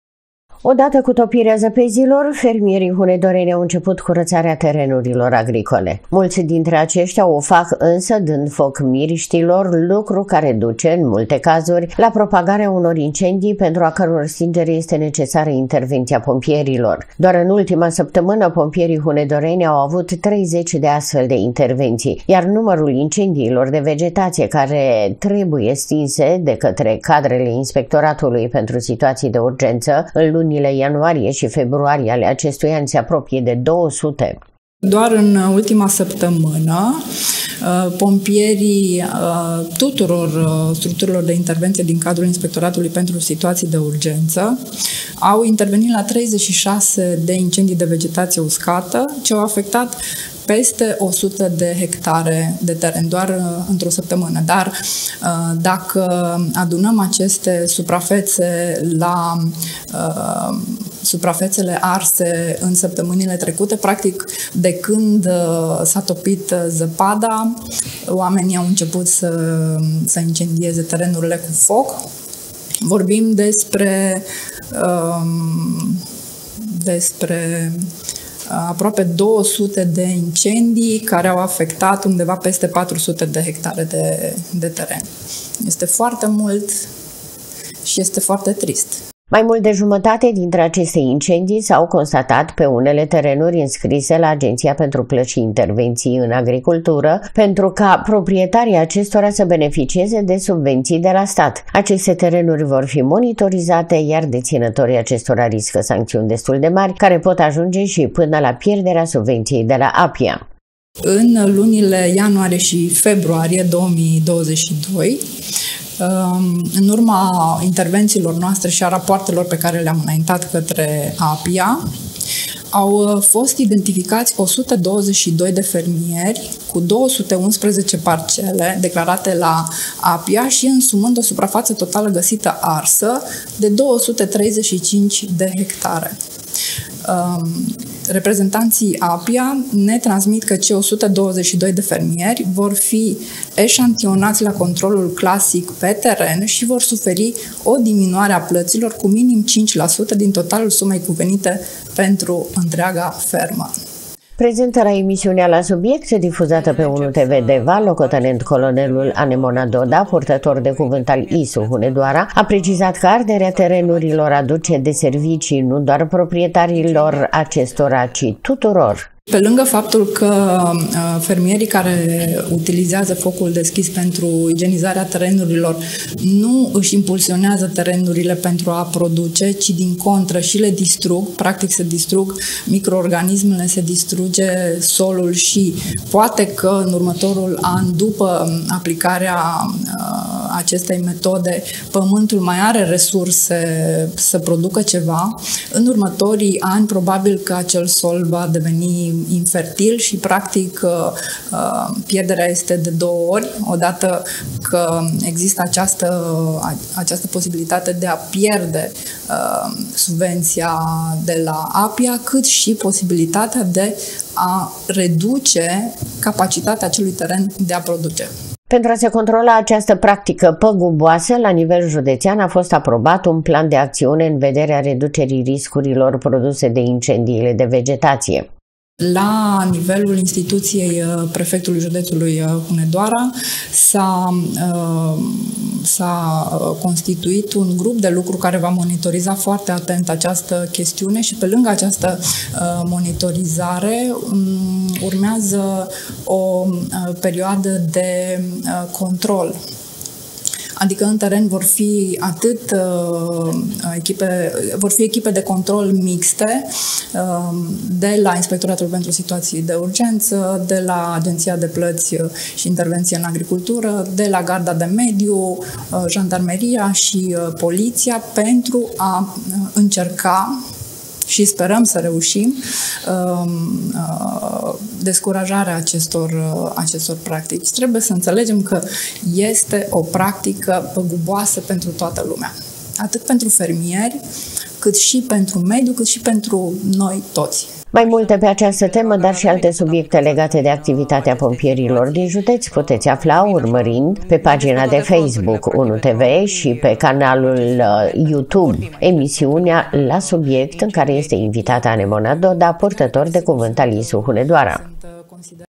APROAPE 200 DE INCENDII DE VEGETAȚIE USCATĂ ÎN DOAR DOUĂ LUNI
Pompierii hunedoreni au fost nevoiți să intervină, în primele două luni ale acestui an, la stingerea a aproape 200 de incendii de vegetație uscată. Mai bine de jumătate dintre acestea au fost pe terenuri înscrise la APIA, proprietarii acestora riscând să piardă subvențiile pe care le primesc de la stat.